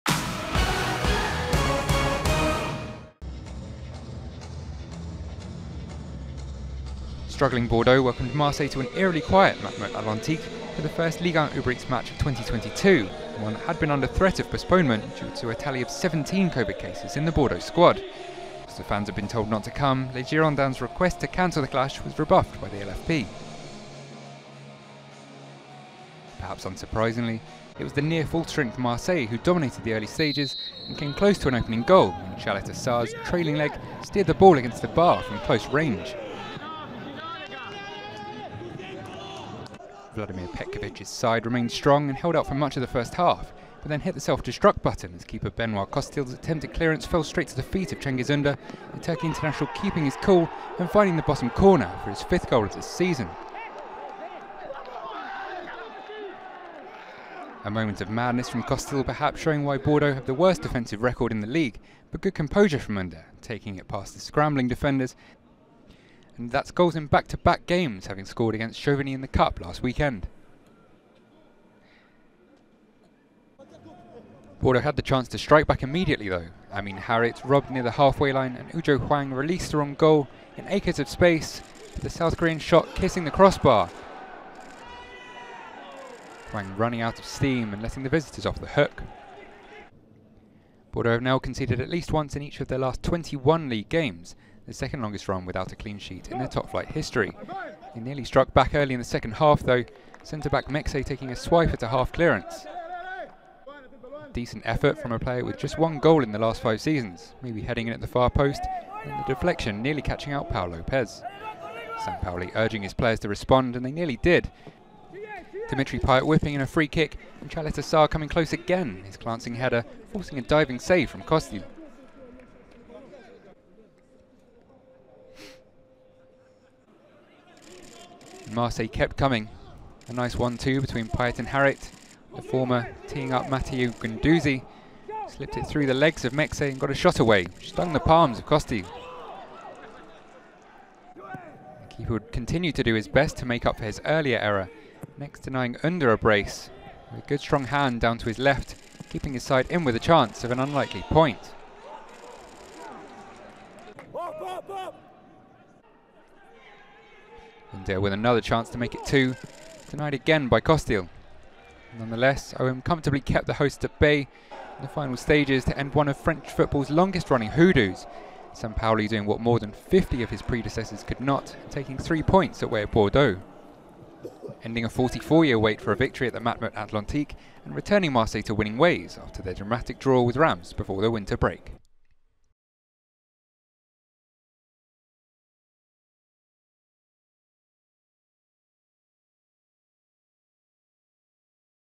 FC GIRONDINS DE BORDEAUX - OLYMPIQUE DE MARSEILLE (0 - 1) - Highlights - (GdB - OM) / 2021-2022
FC GIRONDINS DE BORDEAUX vs OLYMPIQUE DE MARSEILLE Highlights (0 - 1) in video.

Ligue 1 Uber Eats - Season 2021/2022 - Week 20 MATMUT ATLANTIQUE - Friday 7 January 2022

Goals : Cengiz ÜNDER (37' - OM)

FC GIRONDINS DE BORDEAUX Team line-up : 1 - Benoit COSTIL (c) /25 - Enock KWATENG /28 - Rémi OUDIN /4 - Edson MEXER /22 - Timothée PEMBELE /27 - Tom LACOUX /18 - Ui Jo HWANG /20 - Issouf SISSOKHO /29 - Alberth Josue ELIS MARTINEZ /2 - Stian Rode GREGERSEN /21 - Javairo Joreno Faustino DILROSUN

OLYMPIQUE DE MARSEILLE Team line-up : 10 - Dimitri PAYET (c) /7 - Amine HARIT /4 - Boubacar KAMARA /6 - Matteo GUENDOUZI OLIE /2 - William SALIBA /15 - Duje CALETA-CAR /11 - Luis Henrique TOMAZ DE LIMA /29 - Pol Mikel LIROLA KOSOK /17 - Cengiz ÜNDER /16 - Pau LOPEZ SABATA /14 - Luan PERES PETRONI